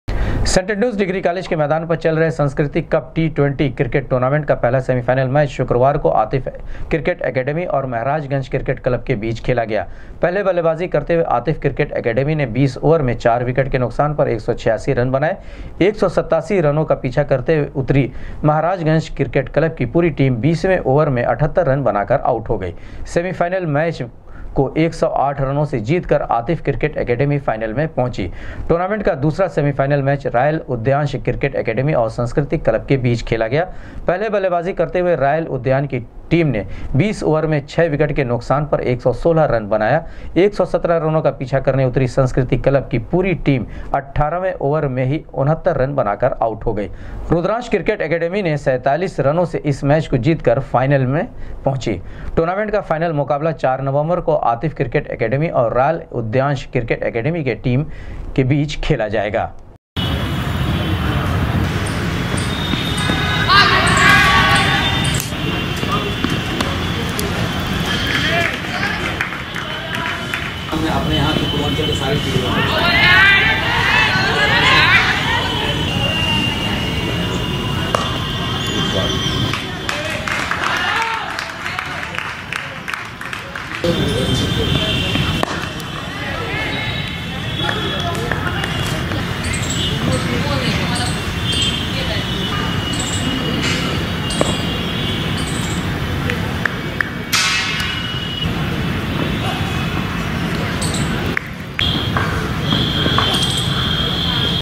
आतिफ क्रिकेट एकेडमी और रॉयल उद्यांश क्रिकेट एकेडमी के बीच होगी, संस्कृति कप टी20 क्रिकेट
आतिफ क्रिकेट एकेडमी और रॉयल उद्यांश क्रिकेट एकेडमी के बीच होगी, संस्कृति कप टी20 क्रिकेट टूर्नामेंट की खिताबी जंग